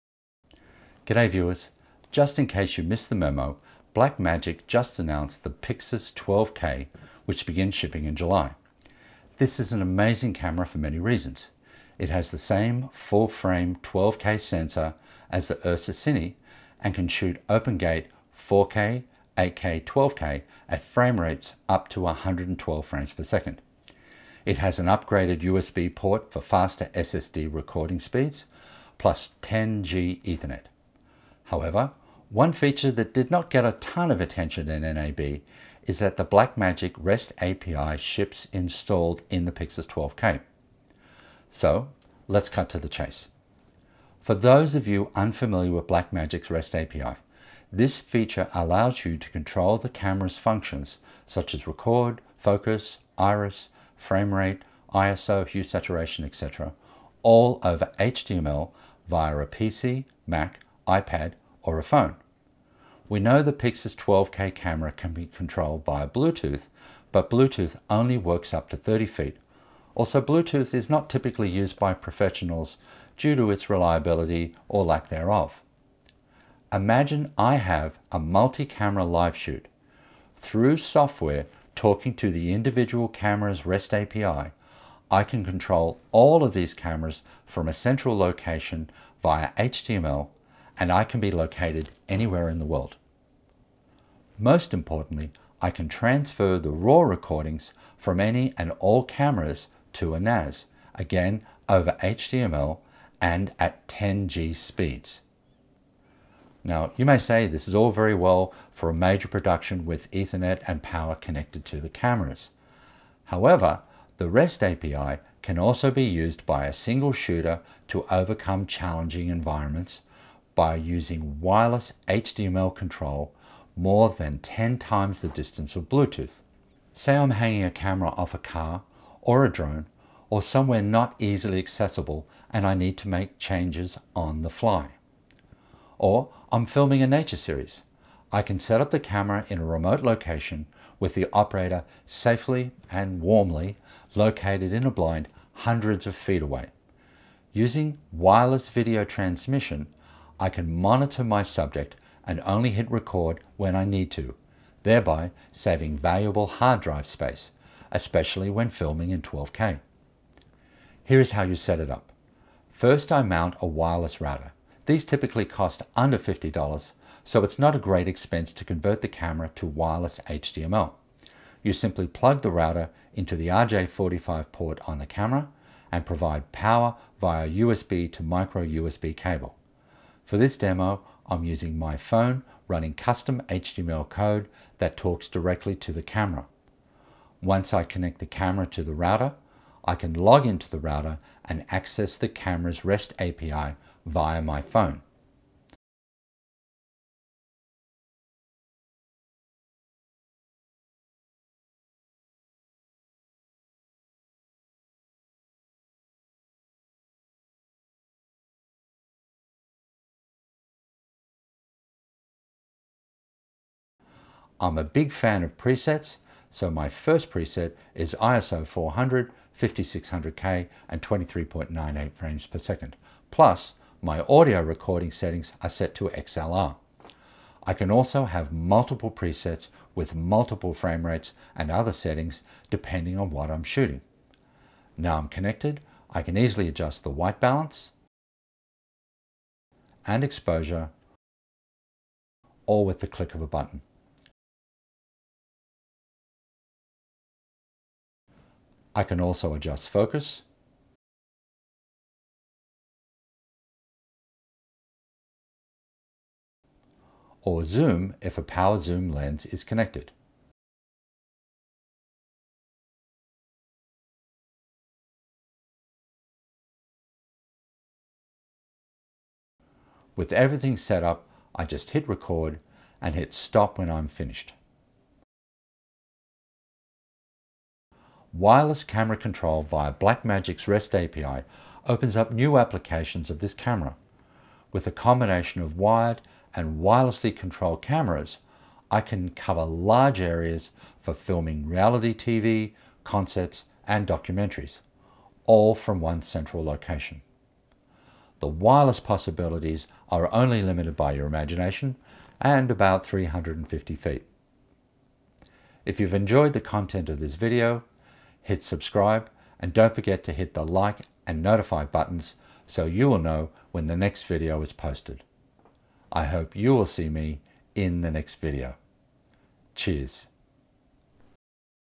Blackmagic PYXIS 12K - REST API Wireless Camera Control over HTML
Blackmagic PYXIS 12K Wireless Control over HTML.  Control a PYXIS 12K via Wireless HTML REST API at 10X the distance of Bluetooth!

Control a Blackmagic PYXIS 12K wirelessly for cameras mounted on a car, on a drone or on the top of a C Stand - Perfect for any application where the camera is inaccessible and you want to control Record/Stop Exposure, ISO etc. from up to 350 feet away!

No warranty is given to the suitability or functionality of any item for a specific purpose.